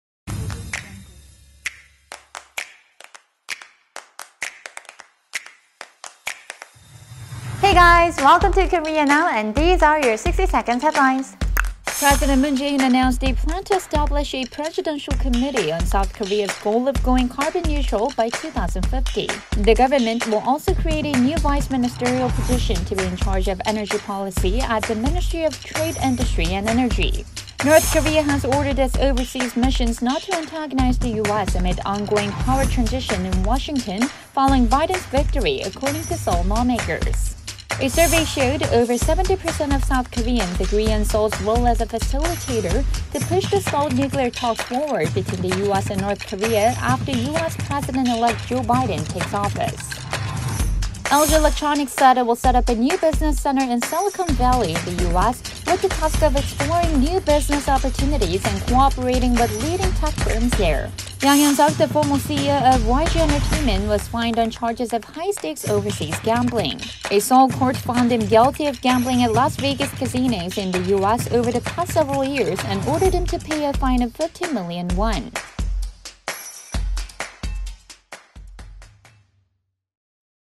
NK overseas missions told not to antagonize US / Seoul as 'facilitator' in nuke talks / YG ex-chief
1. CARBON NEUTRAL

2. N. KOREA

3. NUKE TALKS

4. LG

5. YG chief